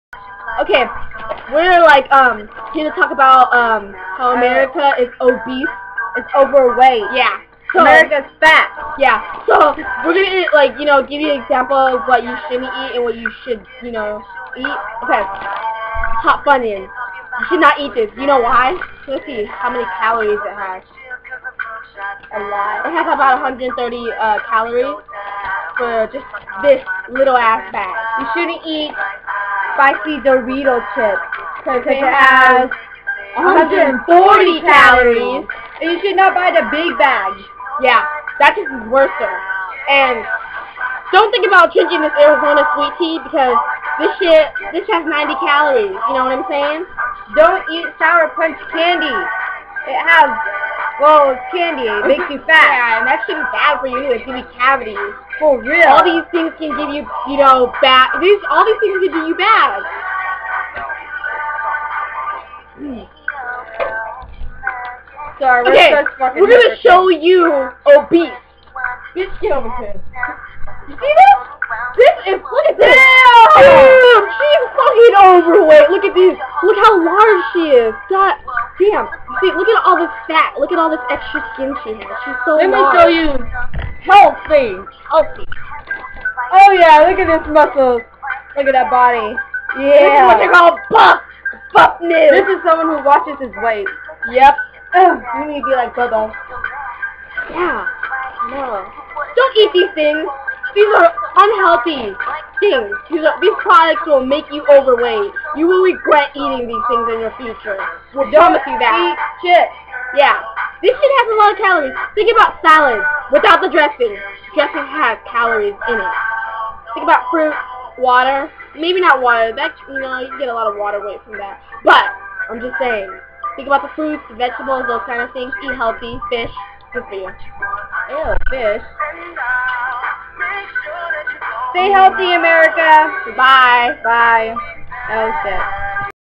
AMERICA'S FAT
people in the US are over weight & . cannot control there size & are eating very unhealthy .